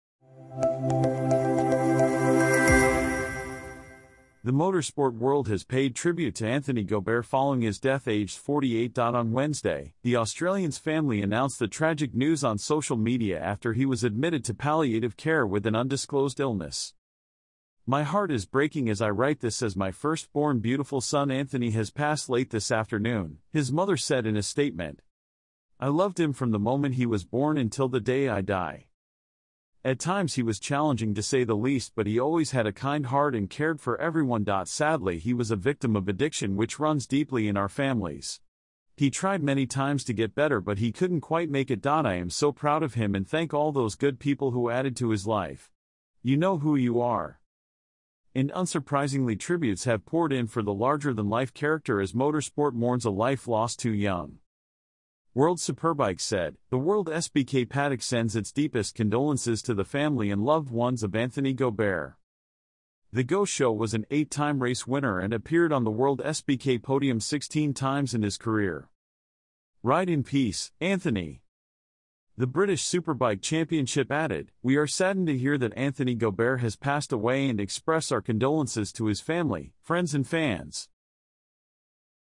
Motorsport legend Anthony Gobert has died after a short illness at the age of 48
The motorsport world has paid tribute to Anthony Gobert following his death aged 48.On Wednesday, the Australian's family announced the tragic news on social media after he was admitted to palliative care with an undisclosed illness.

'My heart is breaking as I write this as my first born beautiful son Anthony has passed late this afternoon,' his mother said in a statement.
'I loved him from the moment he was born until the day I die.

'At times he was challenging to say the least but he always had a kind heart and cared for everyone.Sadly he was a victim of addiction which runs deeply in our families. He tried many times to get better but he couldn't quite make it.I am so proud of him and thank all those good people who added to his life. You know who you are.'

And unsurprisingly tributes have poured in for the larger-than-life character as motorsport mourns a life lost too young.

World Superbikes said: 'The WorldSBK paddock sends its deepest condolences to the family and loved ones of Anthony Gobert.

'The Go Show' was an 8 time race winner and appeared on the WorldSBK podium 16 times in his career.

'Ride in Peace, Anthony.'

The British Superbike Championship added: 'We are saddened to hear that Anthony Gobert has passed away and express our condolences to his family, friends and fans.